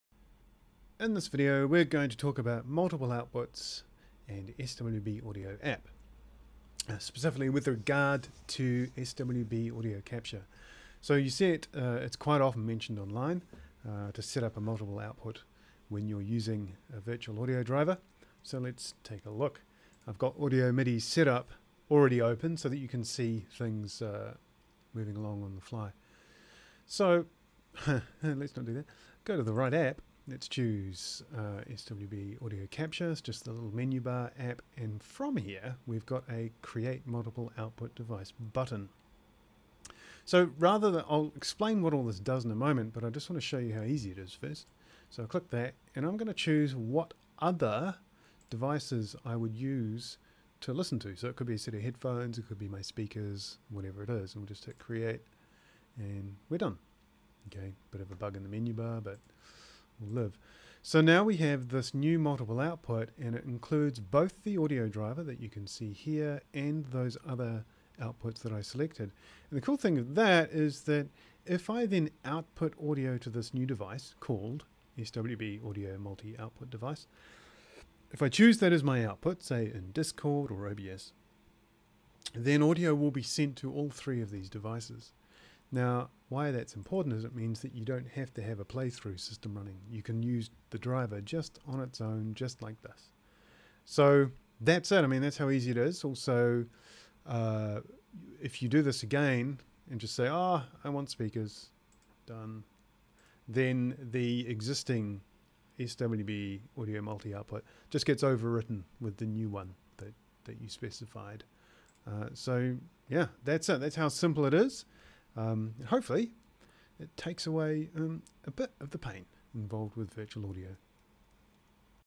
Multiple Output with SWB Audio Capture
How to setup multiple outputs in Audio Midi Setup, via SWB Audio App in 2 clicks.  I also talk a bit about why you'd want to do this.